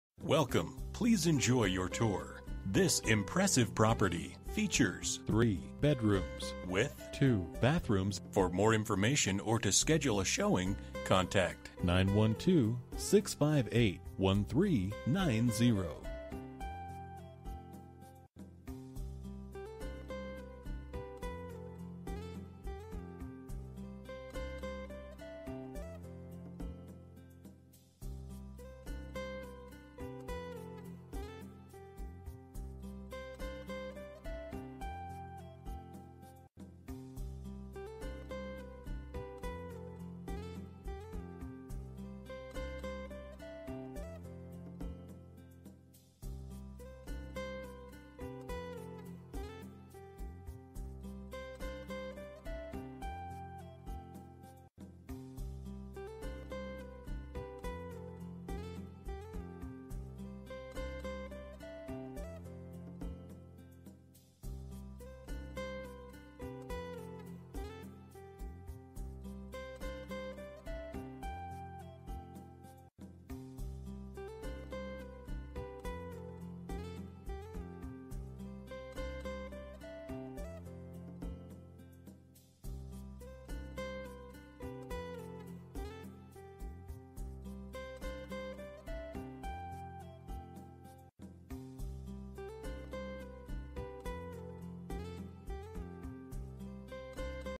1 Quail Forest Ln, Savannah, GA 31419
Move in ready, one story cottage home with all the improvements including upgraded new HVAC, privacy fenced back yard, side courtyard, and outbuilding that remain. Owner has added gutters and a New HVAC. Conveniently located within minutes of two malls, Armstrong Atlantic, Savannah Vo Tech and Hunter Army Airfield. Functional floor plan with vaulted ceiling in the great room and converted garage for separate office or bonus room. Stainless steel appliances in the kitchen. Workshop in back stays. No flood insurance required! City water and sewer!

Bedrooms: 3
Bathrooms: 2
Square Feet: 0
Price:

04/16/2018 07:08:03 pm

Last modified: